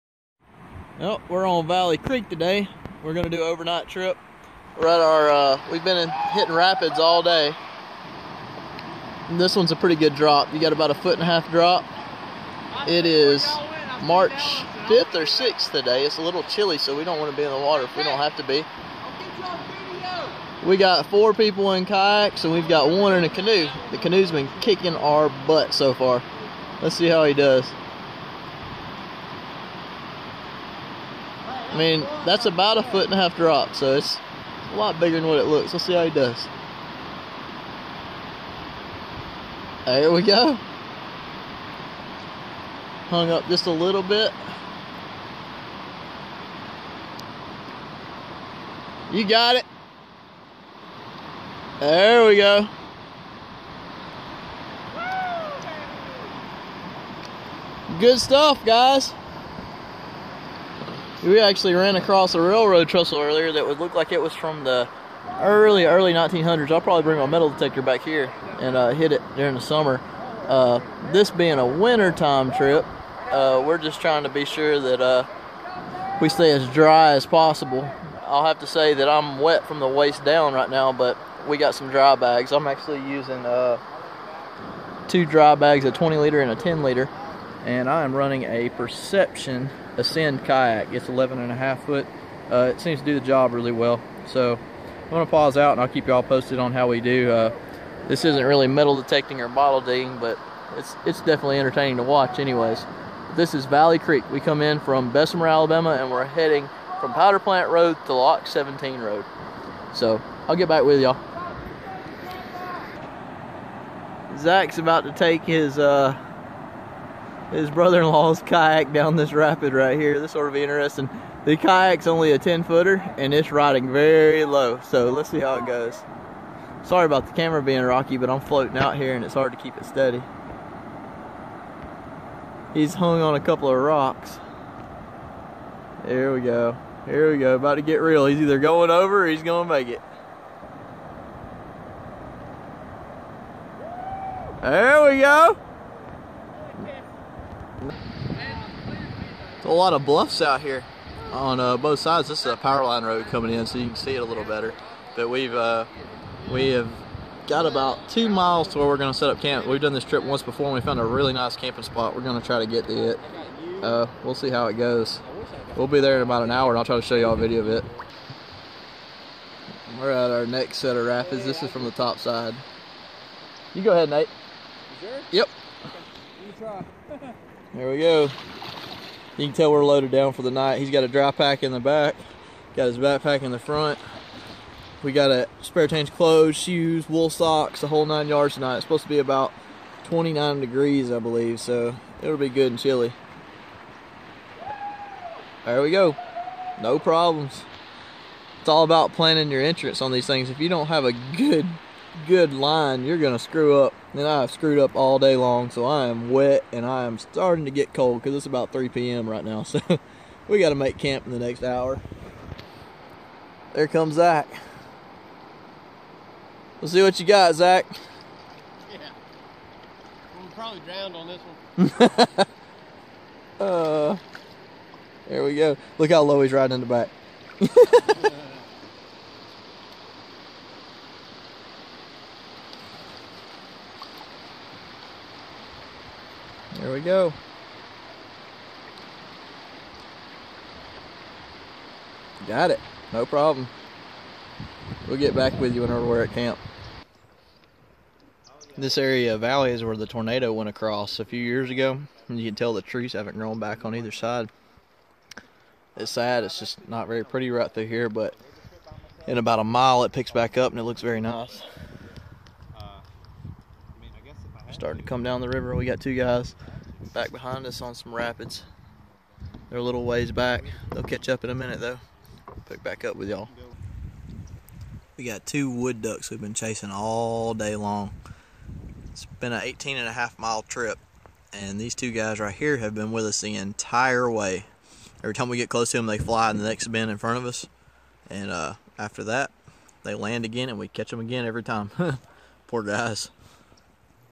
Kayaking Valley creek Bessemer Alabama
Overnight kayak trip in Alabama, March 2017.